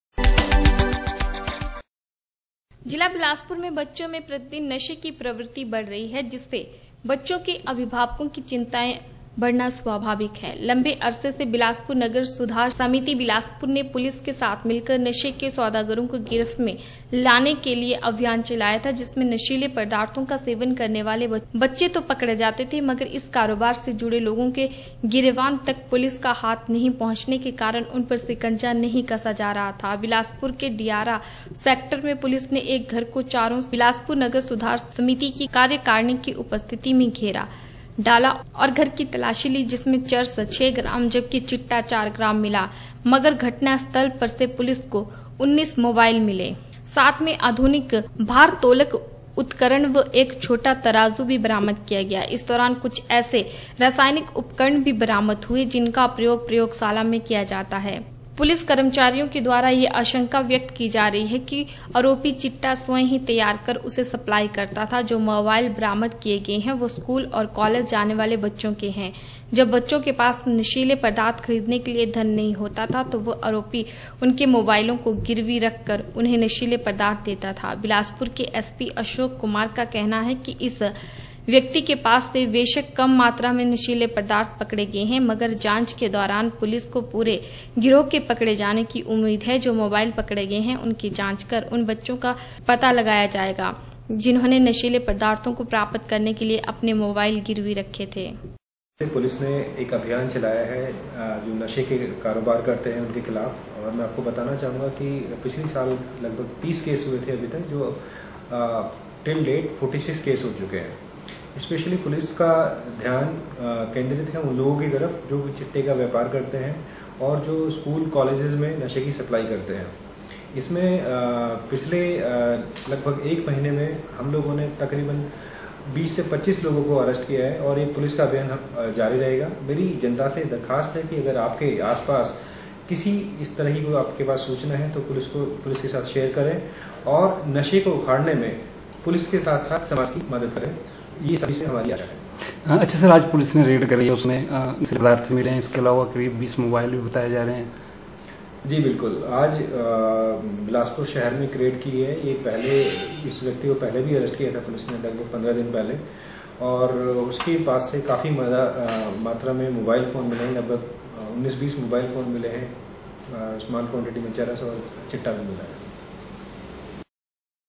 बिलासपुर में बच्चों में प्रतिदिन नशे की प्रवृति बढ़ रही
बिलासपुर में बच्चों में प्रतिदिन नशे  की प्रवृति बढ़ रही है जिससे बच्चों के अभिभावकों  की चिंता बढ़ना स्वाभाविक है। लम्बे अर्से से  बिलासपुर नगर सुधार समिति बिलासपुर ने पुलिस के साथ मिलकर नशे के सौदागरों को गिरफ्त में लाने के लिए अभियान  चलाया था बिलासपुर के डियारा सैक्टर में पुलिस ने एक घर को चारों से बिलासपुर नगर सुधार समिति की कार्यकारिणी की उपस्थिति में  घेरा डाला और घर की तलाशी ली जिसमें चरस छह ग्राम जबकि चिट्टा चार ग्राम  मिला मगर घटना स्थल पर से पुलिस   को उन्नीस  मोबाईल मिले साथ में आधुनिक भारतोलक उपकरण व एक छोटा तराजू भी बरामद किया। इस दौरान  कुछ ऐसे रासायनिक उपकरण भी बरामद हुए हैं